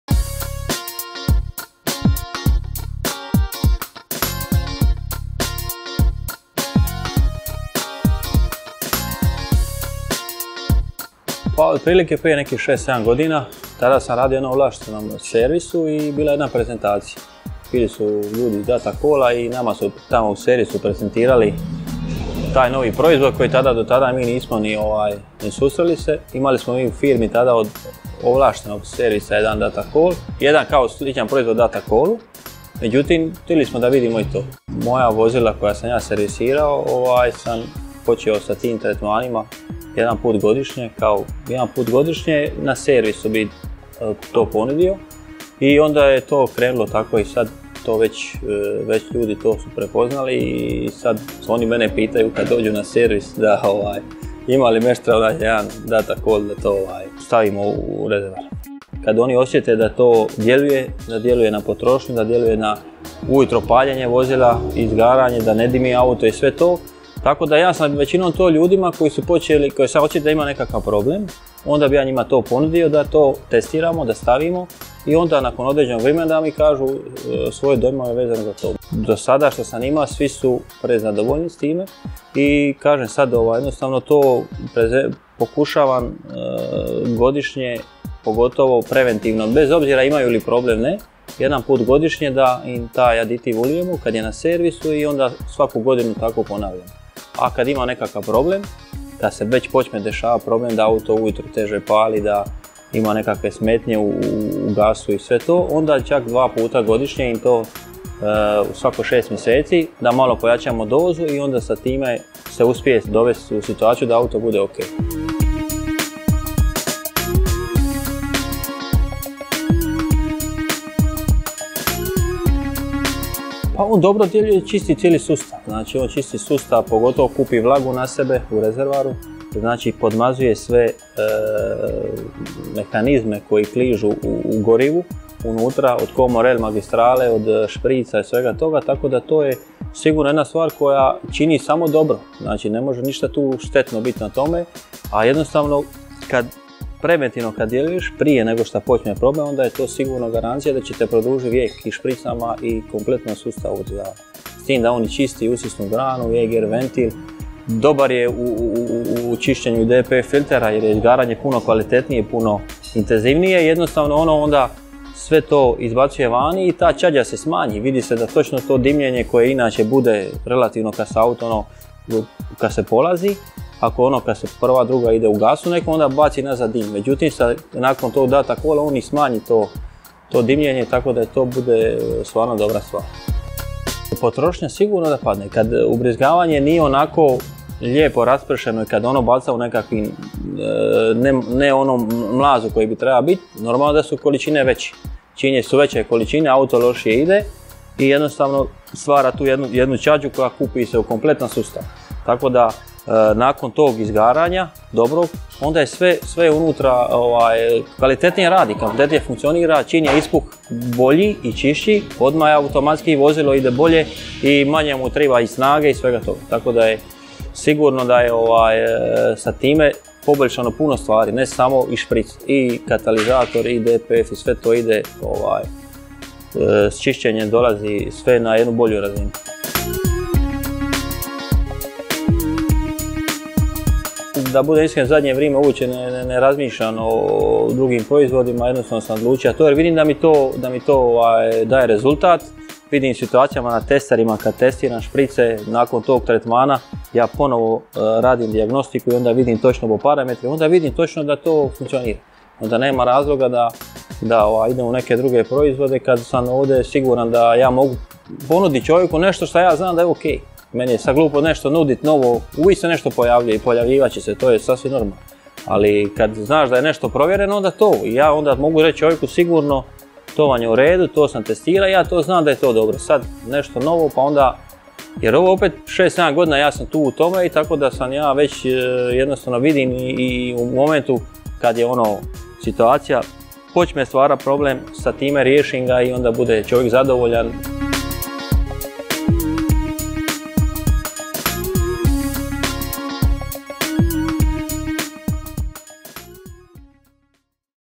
Pitamo profesionalce - Kako poboljšati performanse i učinkovitost vozila?
Koliko često, kako djeluju i na koji način koristiti Datacol aditiv... samo su neka od pitanja koje smo upitali profesionalce.